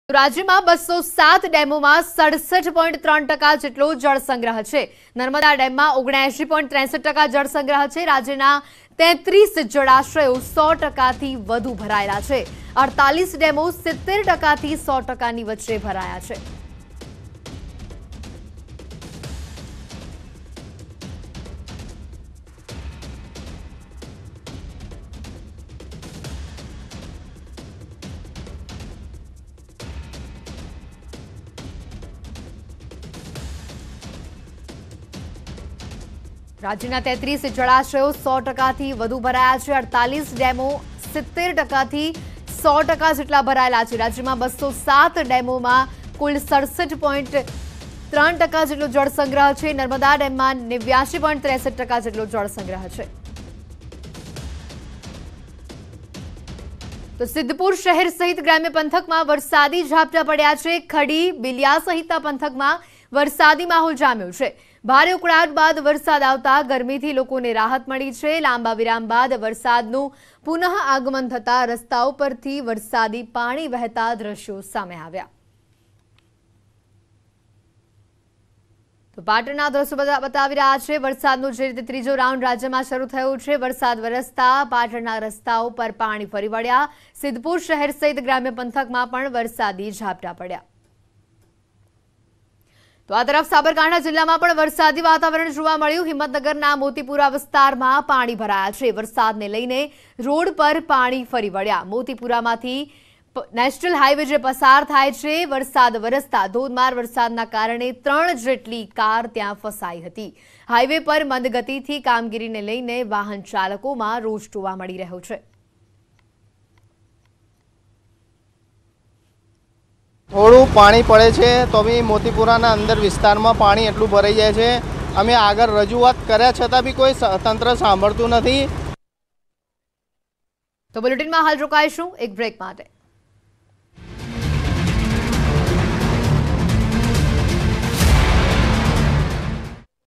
Gujarat weather: રાજ્યના 207 ડેમમાં 67 ટકા જળ સંગ્રહ | Gujarat News | News18 Gujarati
Reservoirs overflowed with good rains in the state. 67 percent water storage in 207 dams. On the other hand, 79 percent of water bodies in Narmada Dam.

રાજ્યમાં સારા વરસાદથી જળાશયો છલકાયા. 207 ડેમમાં 67 ટકા જળ સંગ્રહ. તો આ તરફ નર્મદા ડેમમાં 79 ટકા જળસંગ્રહ.n18oc_gujarat